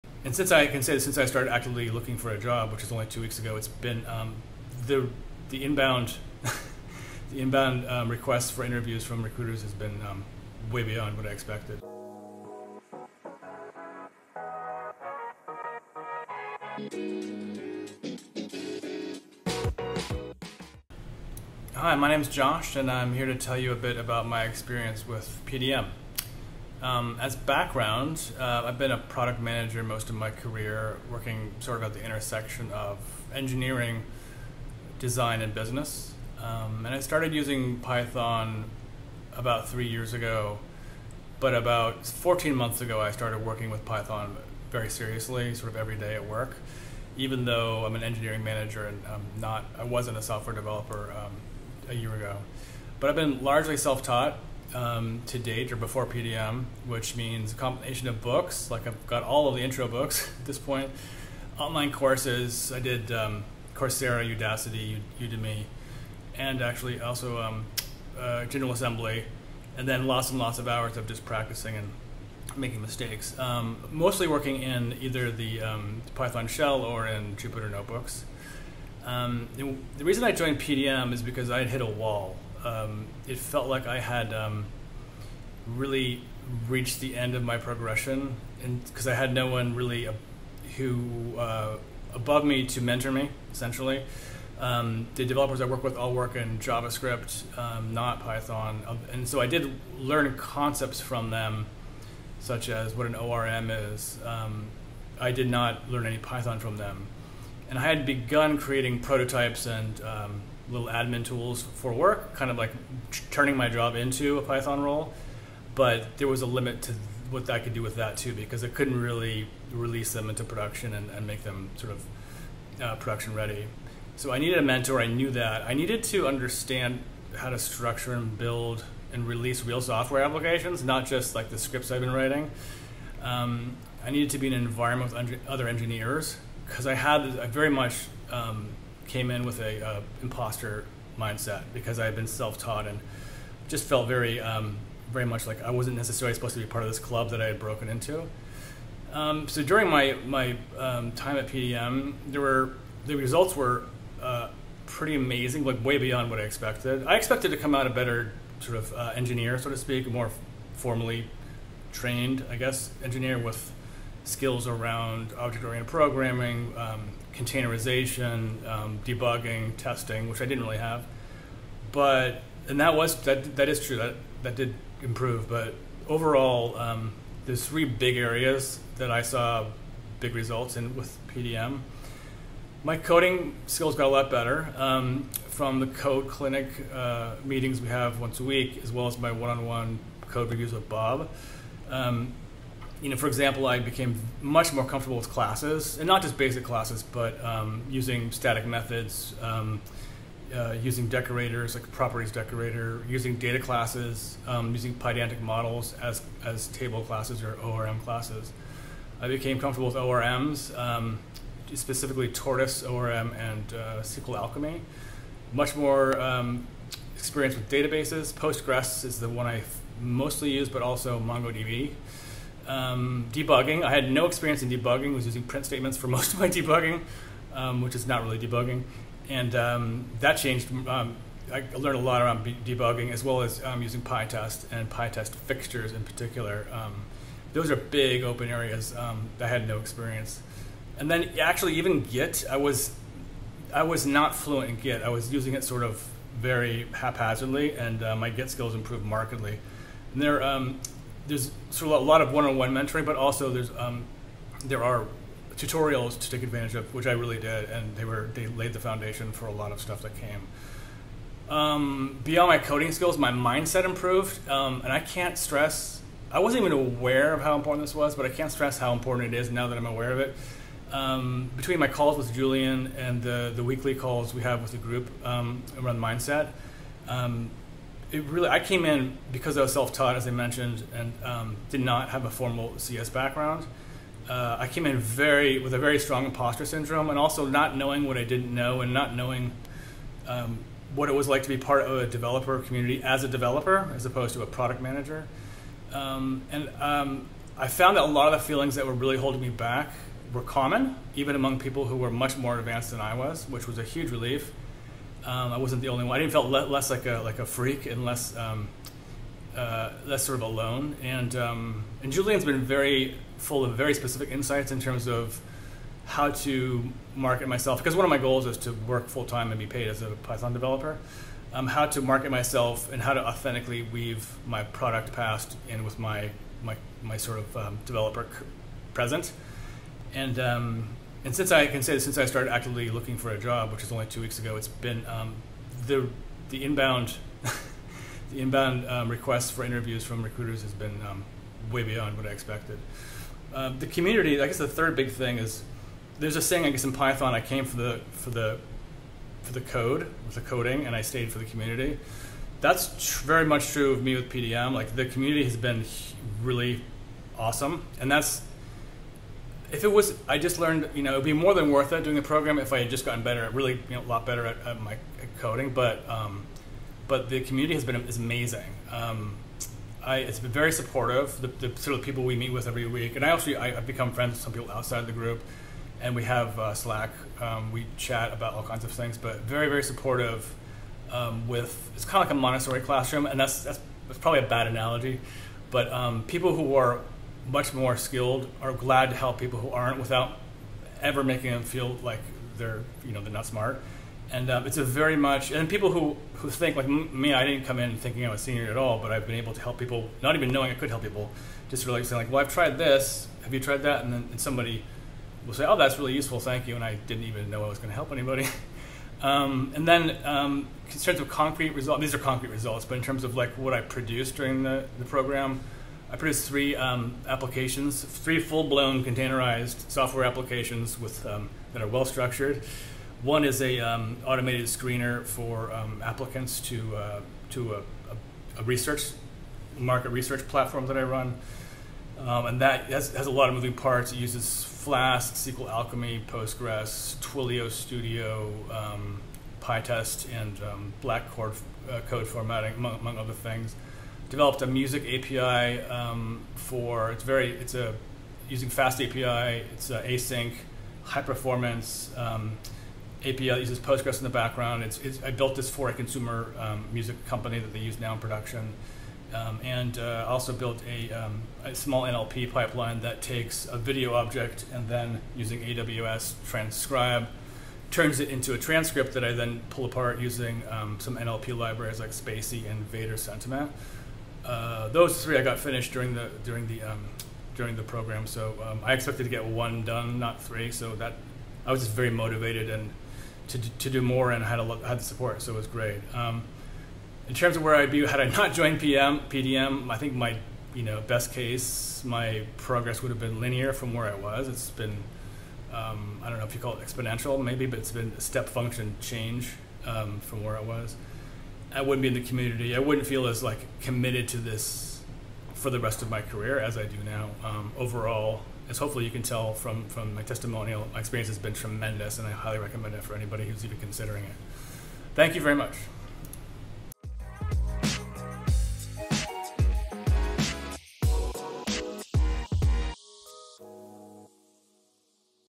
How Josh accelerated his Python journey through PDM
Today on the channel Josh's story / transformation he experienced through our PDM (Pybites Developer Mindset) program.

Chapters:
00:00 Intro
00:20 Meet Josh + his background
00:53 How Josh initially learned Python
01:20 Reasons why Josh joined PDM
02:30 Josh's time in PDM, what he got out of the program
03:00 Key Python + developer skills Josh learned with us
04:12 PDM's coaching format + the importance of mindset
04:57 Feelings that held him back were actually common 💡
05:15 PDM helped me position myself much better as a developer 💪
05:50 The PDM community has been amazing + supportive 😍
06:53 The 3 apps Josh produced in the PDM program 🔥
08:07 Where I would be had I not joined PDM + final words
08:37 Outro

Ready to take your Python journey to the next level?